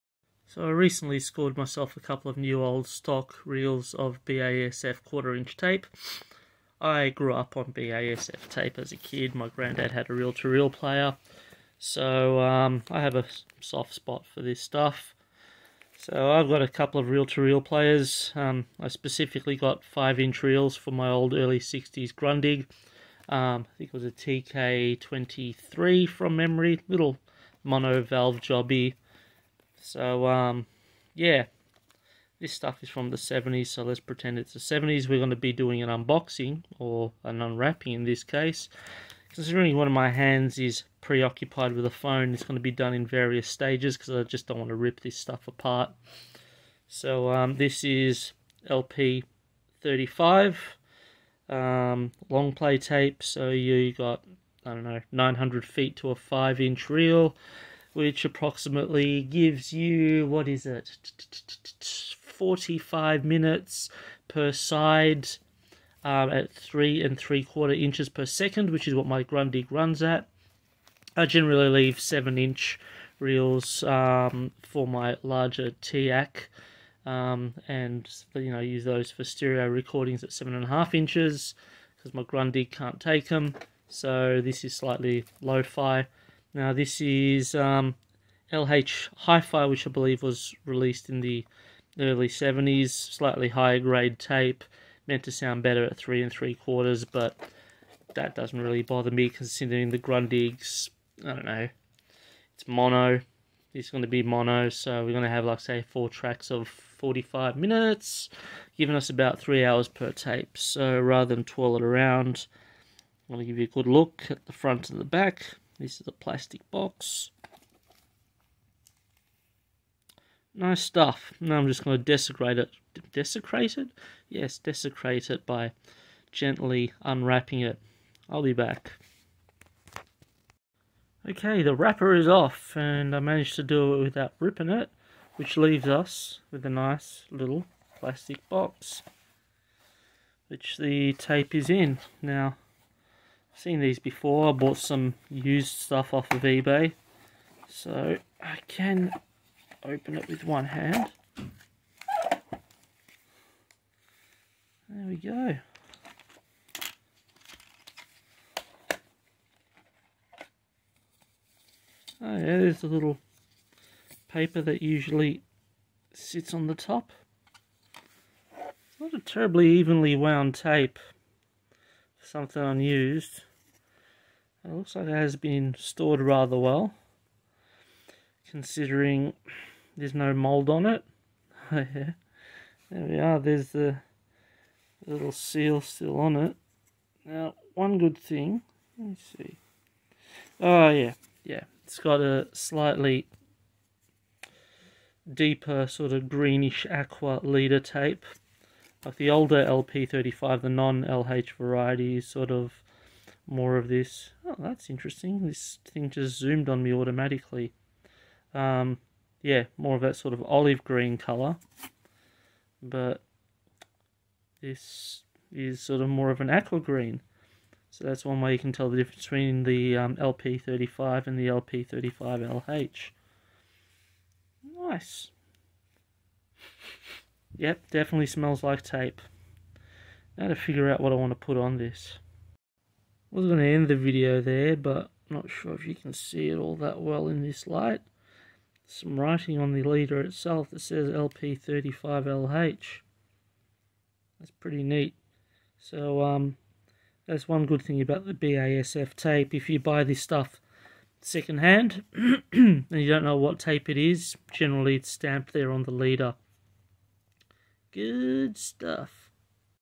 Opening A New Old Stock Reel Of BASF LP35LH Tape
Bought myself a couple of unopened new old stock 5" reels of BASF LP35LH quarter inch tape and I decided to make a video. It's not every day that a person can experience what it would be like to open a fresh untouched reel of audio tape made in the 1970's, so I figured I'd share that experience regardless of how unexciting it may be to a lot of people. Hell, this is YouTube so there are videos for every niche interest. Here's another!